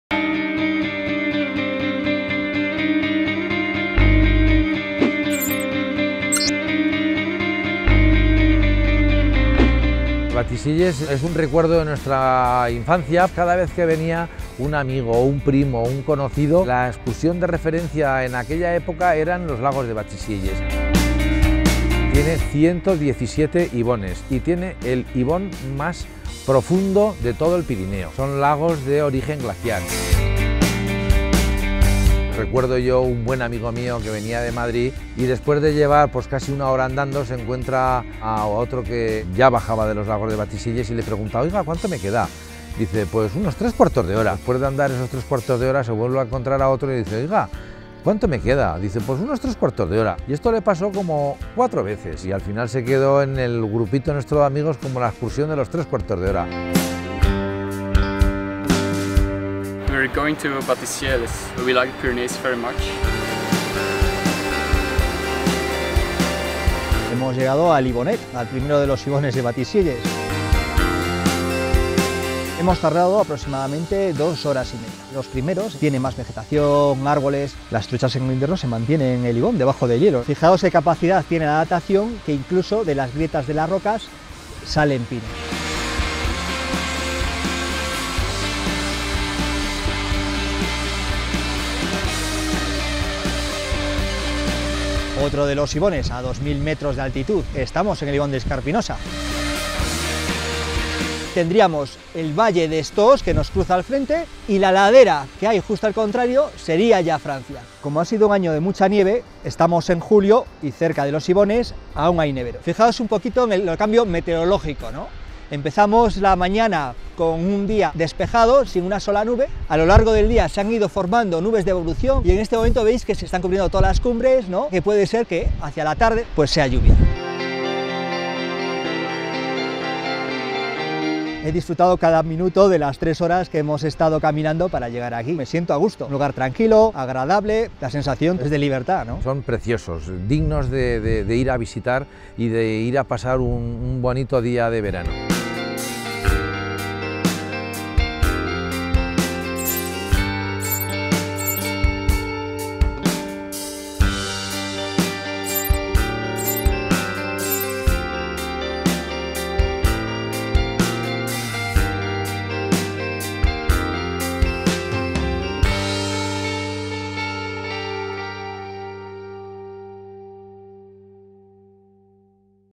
RincónBatisielles, candidato a El Mejor Rincón 2013 por Aragón
Los Ibones de Batisielles, candidatos a El Mejor Rincón 2013 para la Guía Repsol